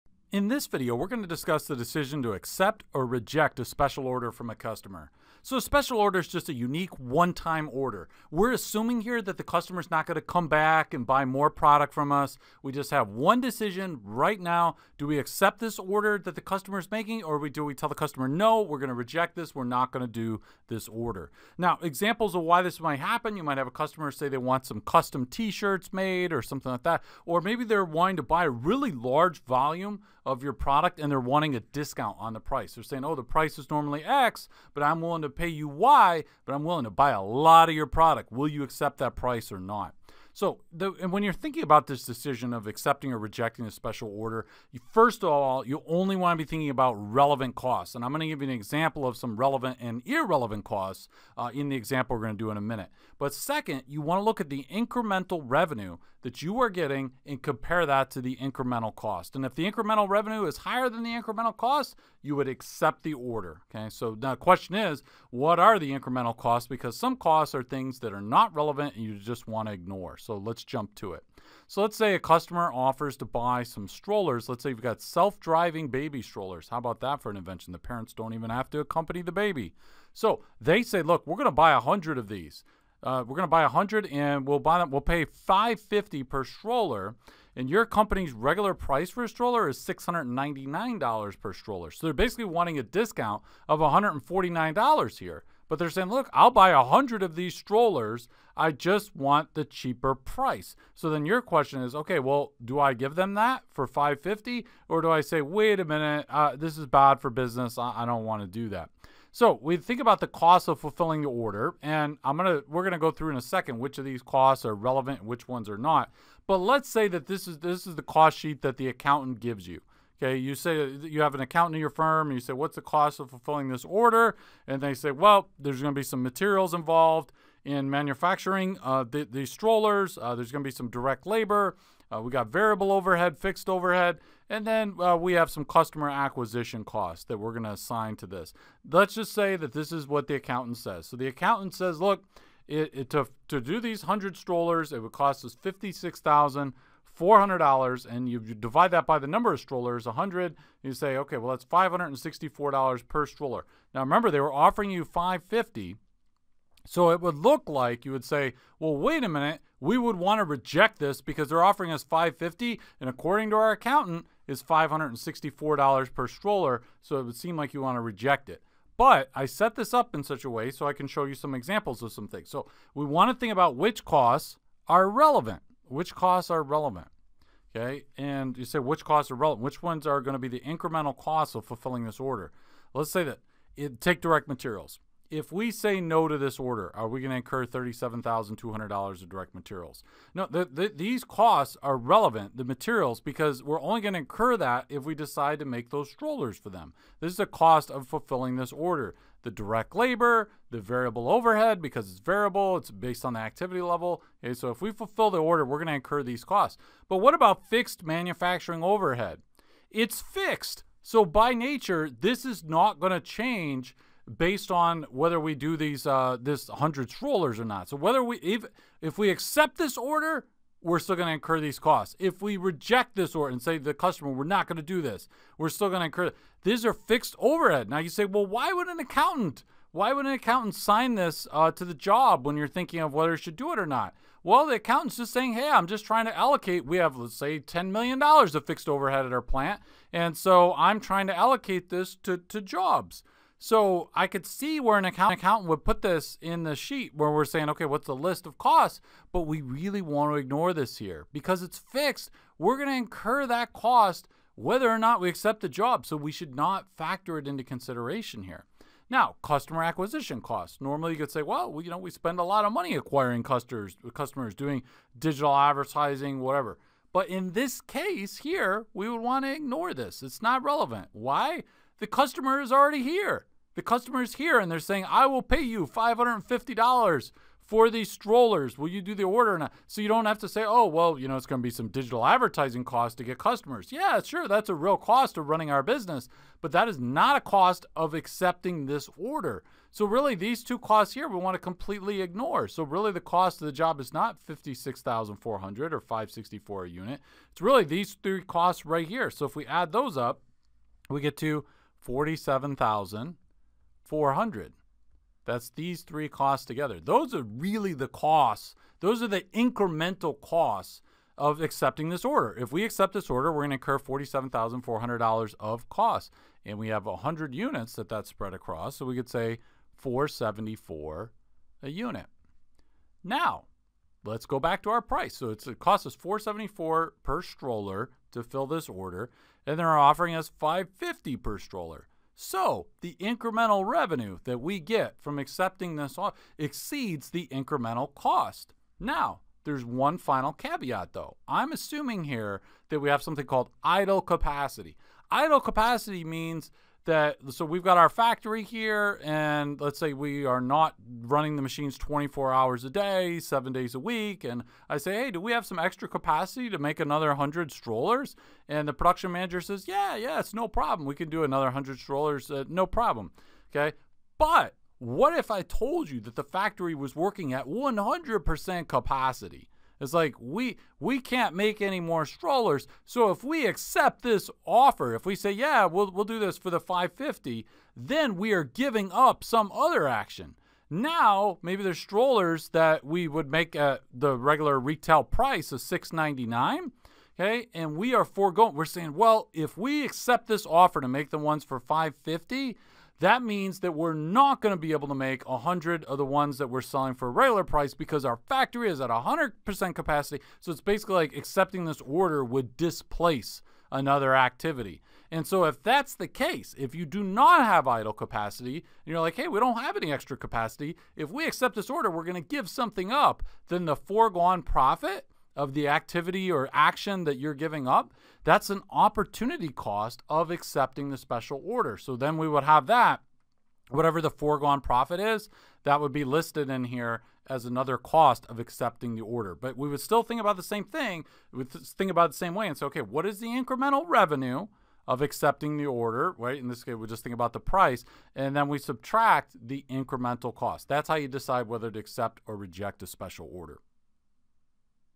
Accept or Reject Special Order | Managerial Accounting
When deciding whether to accept or reject a special order, you should:
1. only consider relevant costs (ignore any costs that won't change whether or not you accept the order)
2. accept the order if the incremental revenue exceeds the incremental cost of accepting the order

If you don't have idle capacity, then the forgone profit of any actions you are giving up (by accepting the special order) should be included as a cost of accepting the special order.—
ACCESS INDEX OF VIDEOS
CONNECT WITH EDSPIRA
CONNECT WITH MICHAEL
ABOUT EDSPIRA AND ITS CREATOR